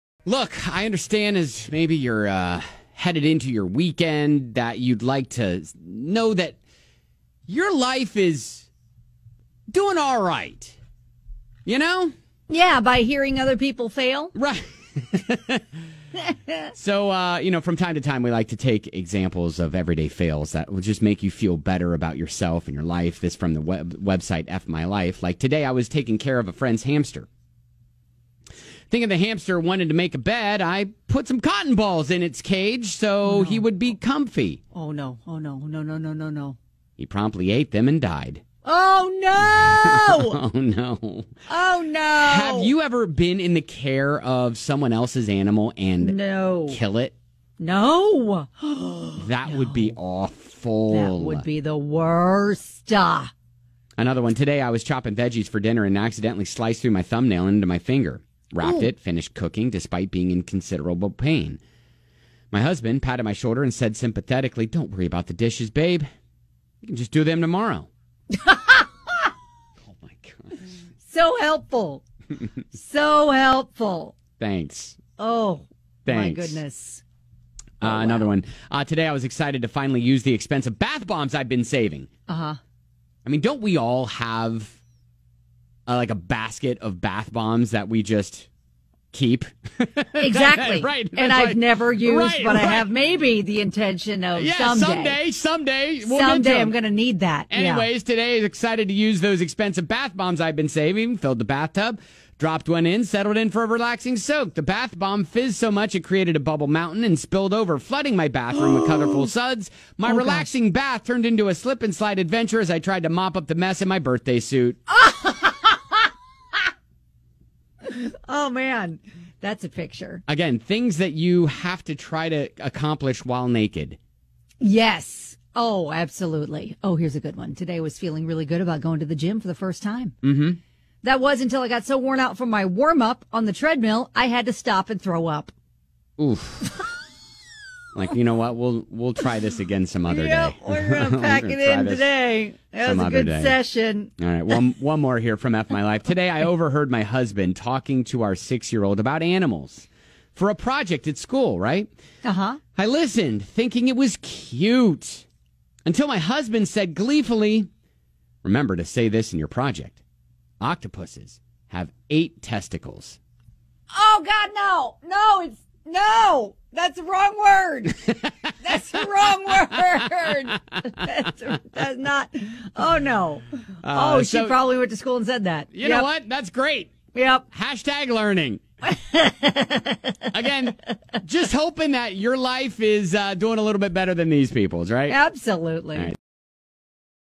F My Life - Octopuses Have 8 WHAT?!?!
Here are some examples of everyday fails that will make you feel better about yourself and your life! From the website “F My Life”